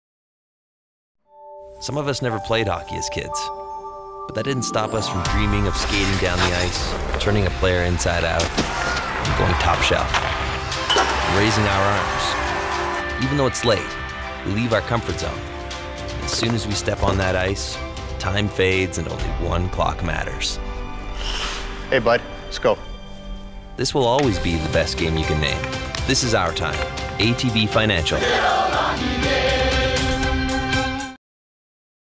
ATB - Our Time
One of the television spots we produced this year for ATB Financial. Ads featured the Calgary Flame Mark Giordano and Edmonton Oiler Jordan Eberle.